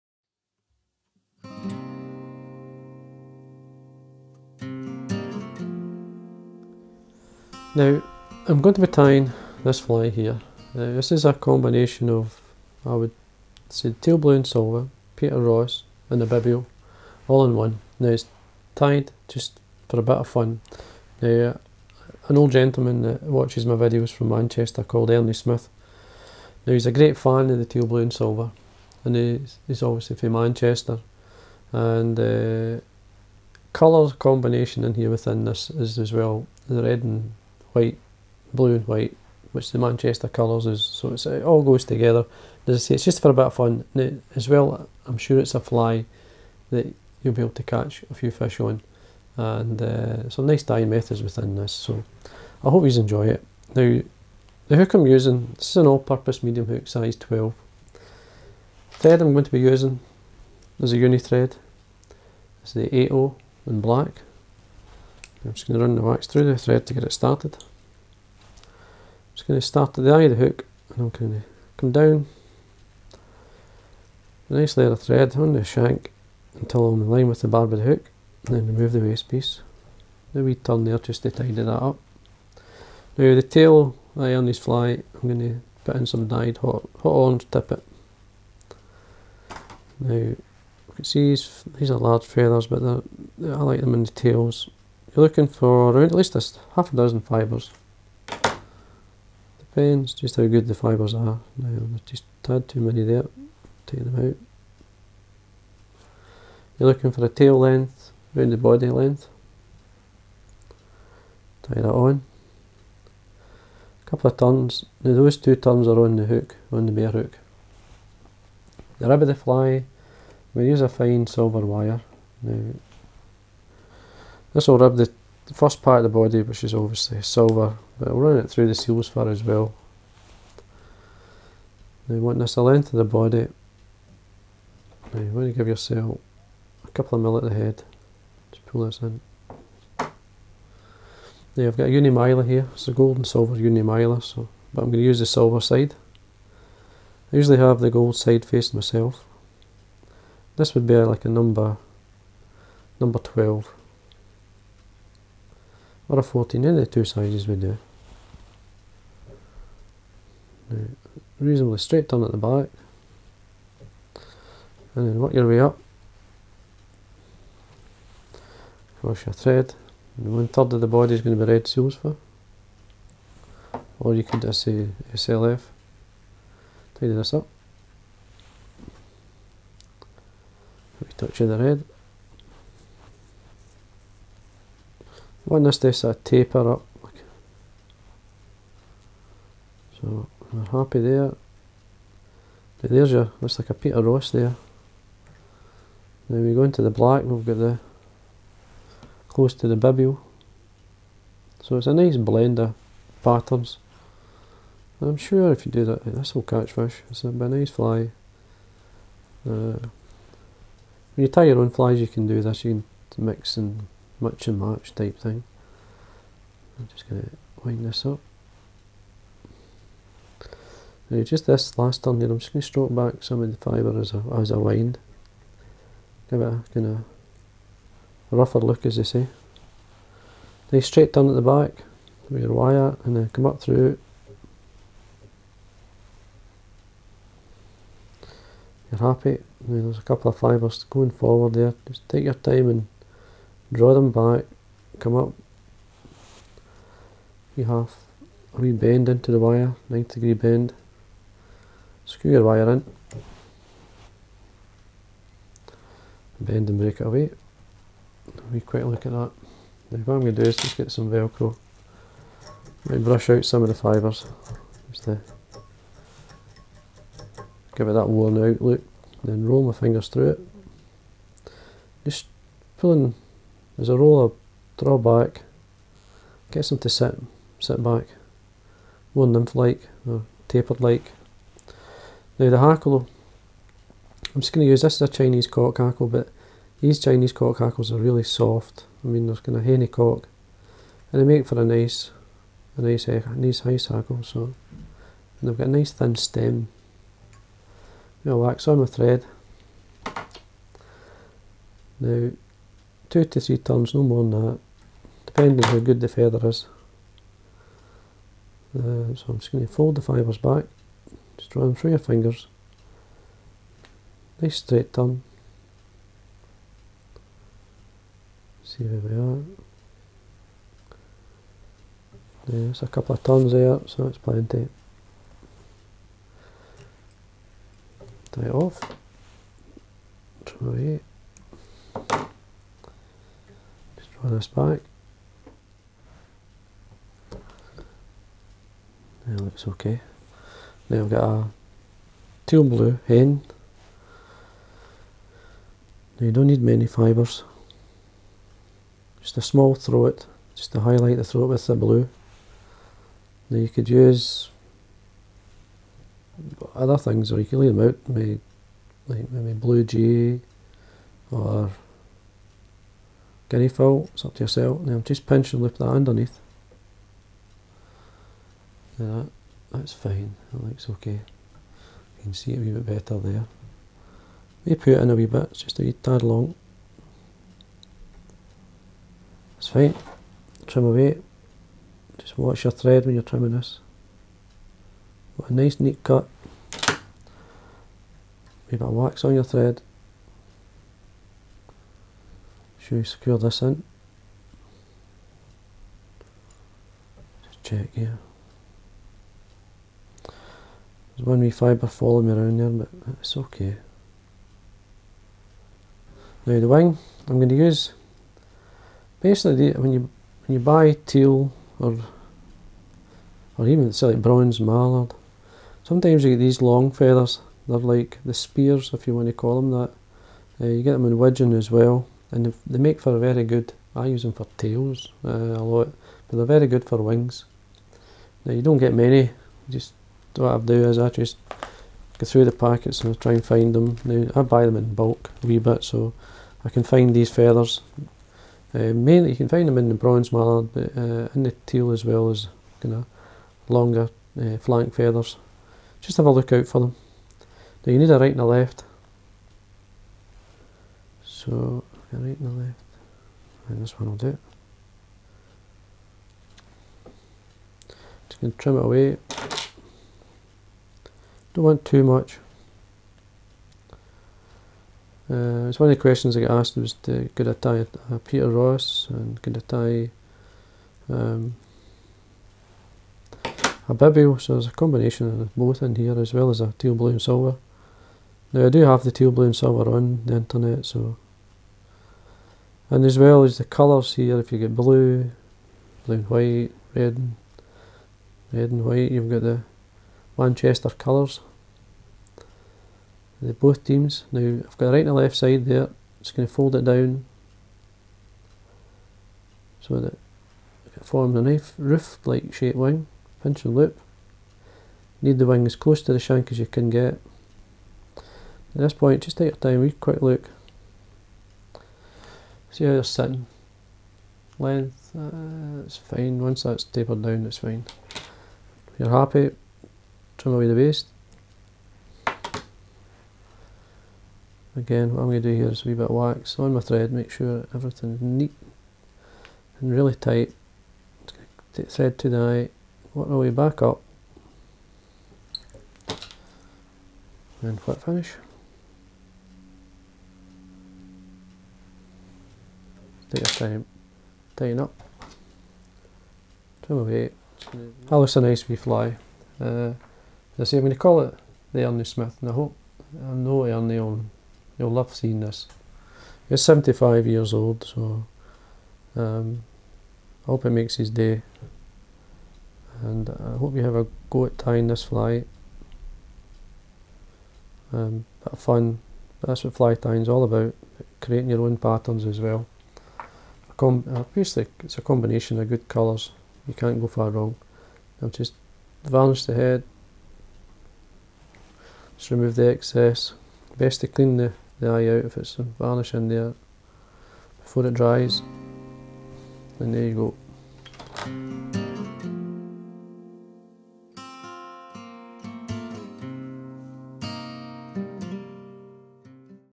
Tying the Ernie Smith (Wet Fly) by Davie McPhail
The Ernie Smith Wet Fly

Materials Used;
Hook, Fulling Mill All-Purpose Medium size 12
Thread, Uni-8/0 Black
Tail Golden Pheasant Tippet dyed Hot Orange
Rib, Silver Wire
Body, Silver Tinsel and Red and Black Seals Fur or Sub in Equal Parts
Hackle, Black
Throat, Teal Blue Hen
Wing, Teal or Widgeon Flank
Hi Everyone,
I've named this fly The Ernie Smith after a 75 year old gentleman from Manchester who is a big fan of my videos on youtube..He visits his local library to view my videos and friends also allow him to watch them on their tv in their home when he's there even though they don't have any interest in fishing but they see how much Ernie enjoy's watching them..His favourite fly is the Teal Blue and Silver and as I already have this on Youtube I decided to combine the Teal Blue and Silver with the Peter Ross and a wee touch of a Bibio to come up with the Ernie Smith which I'm sure he will be very happy to see..I'm sure this will be a great fishing fly as you can't go wrong with these colours..All the very best Davie...